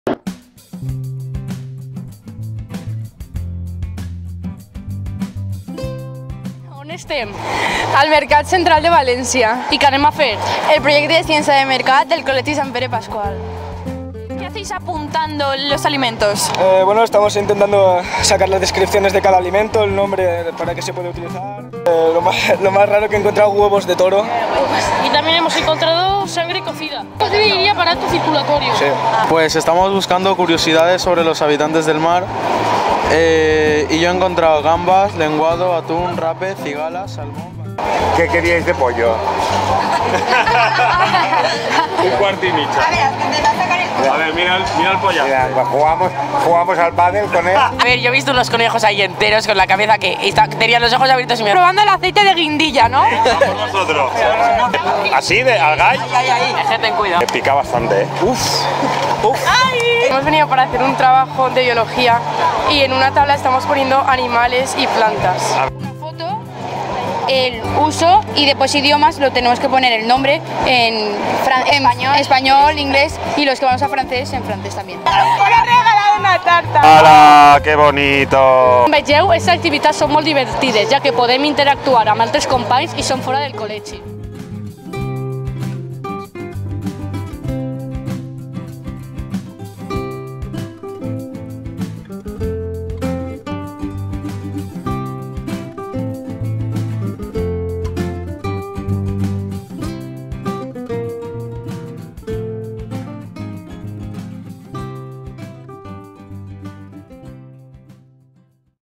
CienciadeMercado MercatCentral mercadocentral biología alimentos 19-20
Experimentando con los alimentos 19-20

El 14 de febrero nos fuimos de visita al Mercado Central para realizar el trabajo de Biología sobre la anatomía de animales y plantas. Gracias a esto hemos aprendido más cosas del tema de forma práctica. La experiencia nos ha encantado y además de aprender, también cominos muchas cosas que estaban riquísimas.

Proyecto CienciadeMercado de la @generalitatvalenciana